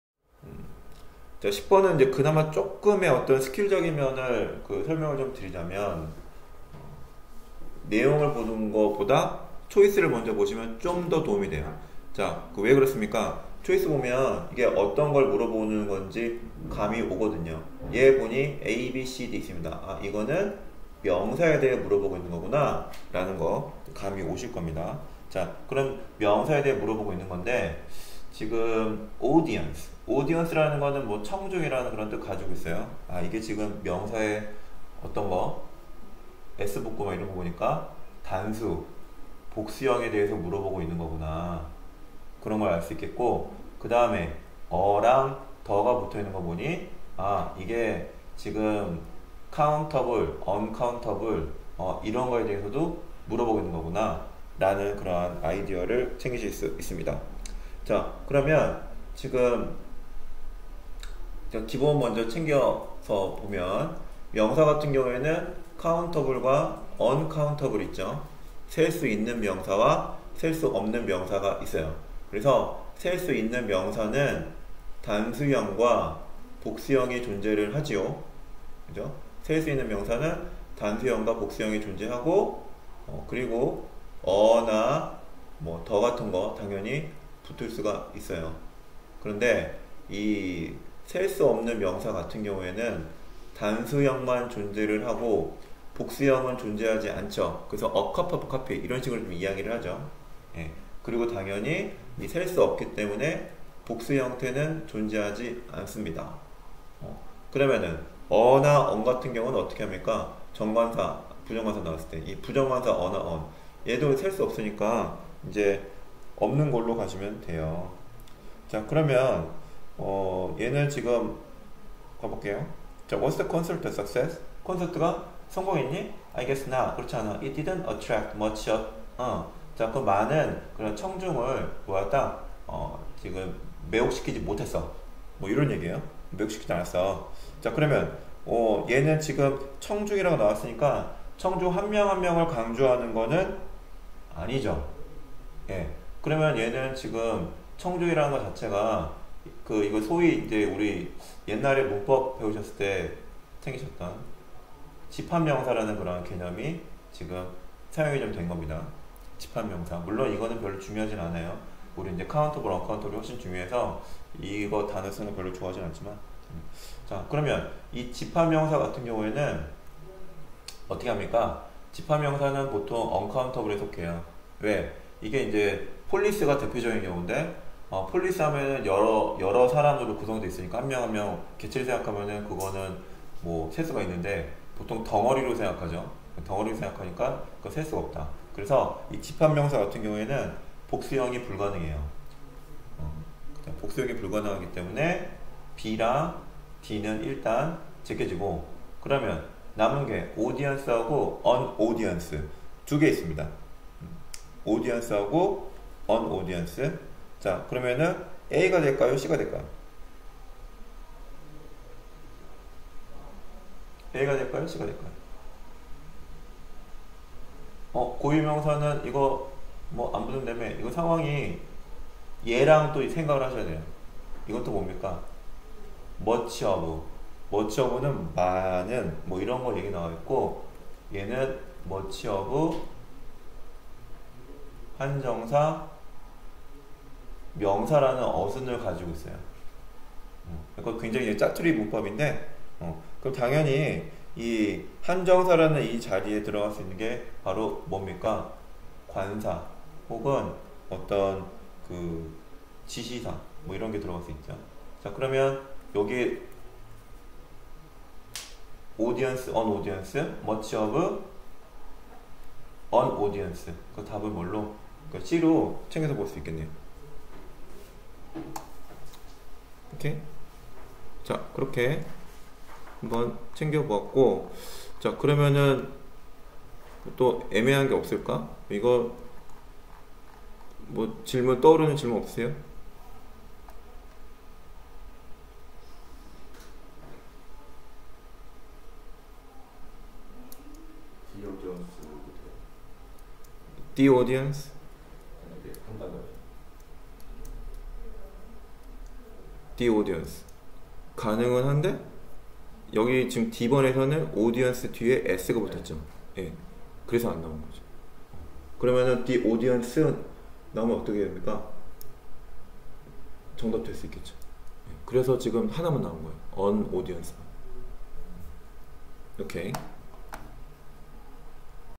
텝스 문법 파트 1 Q 10 A 2 Day 05 곽영어
다니엘샘의 뉴텝스 모의고사 A 2 해설강의입니다.